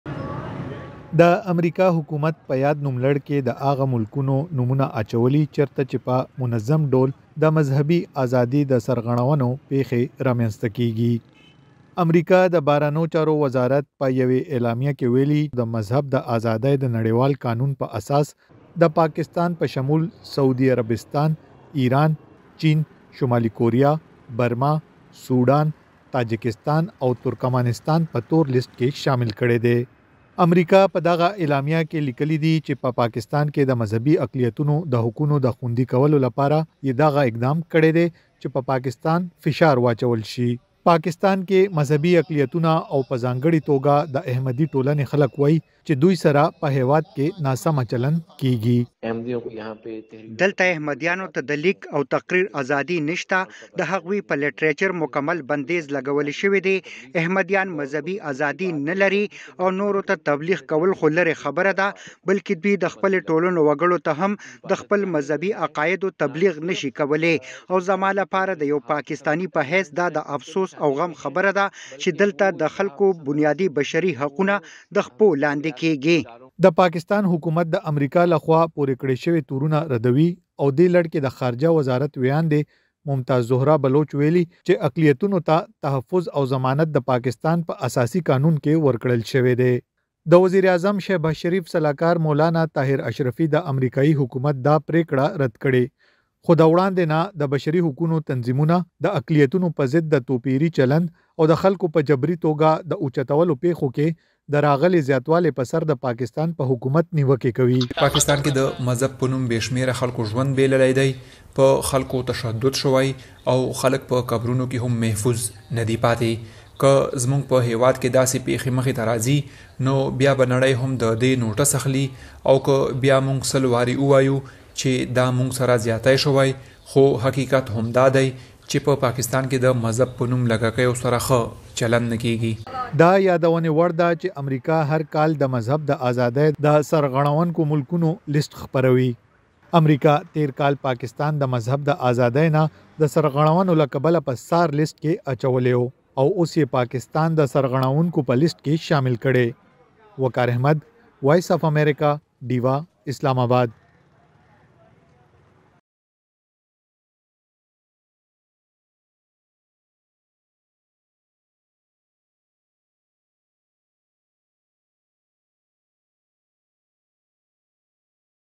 امريکې پاکستان د مذهبي آزاديو د سرغړونو په اړه د ځانګرې اندېښنې وړ هيوادونو کې شامل کړی
امريکې پاکستان د مذهبي آزاديو د سرغړونو په اړه د ځانګرې اندېښنې وړ هيوادونو کې شامل کړی. پاکستان کې لږکي تور پورې کوي چې مذهبي آزادي نه لري خو د پاکستان حکومت دا تورونه ردوي او وايي په هيواد کې د لږکیو حقونه خوندي دي. تفصیل د اسلام آباد نه د وقار احمد په رپوټ کې: